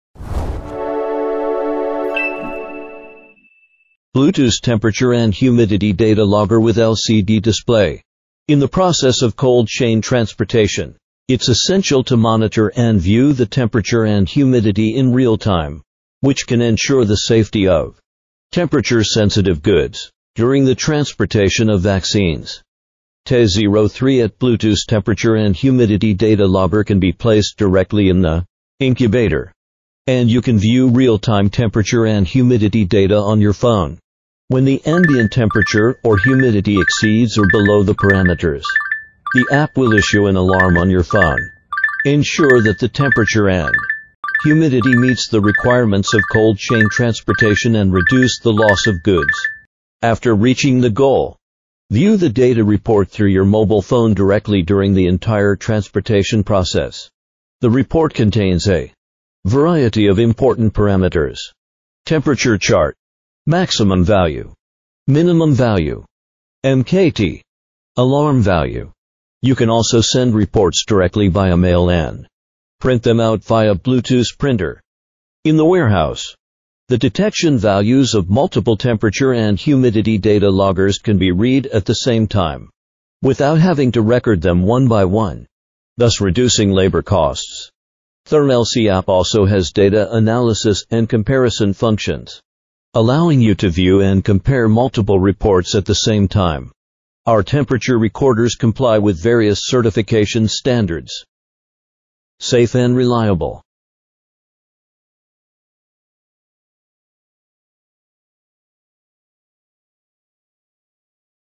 TE-B3 TH Bluetooth Temperature Humidity Data Logger: Wireless Setup, High Accuracy, Real-Time Alerts
Monitor your temperature-sensitive goods with ease using our Bluetooth Temperature & Humidity Data Logger! 🌡️
This smart device connects wirelessly via a mobile app, allowing you to view real-time data, download PDF/CSV reports, and simplify your workflow—all without the need for a computer. Perfect for cold chain management, warehouses, pharmacies, and more!

Key Features:
✅ Wireless Setup & Download: Convenient app setup, real-time data viewing, and instant report export.
✅ Multiple Device Data Comparison: Analyze and compare data from multiple devices to monitor different locations and times.
✅ Visual LED & App Alerts: Instant temperature threshold alerts via LED flash and app notifications.
✅ High Precision Sensor: Accurate temperature range of -20°C to 60°C and humidity 0% to 100% RH.
✅ CFR Part 21 Compliance: Meets industry standards for enhanced accuracy and safety.
✅ Wide Application: Ideal for cold chain management, food processing, museums, hospitals, and more.

Ensure the safety and compliance of your sensitive goods with a Bluetooth data logger that does it all. 📊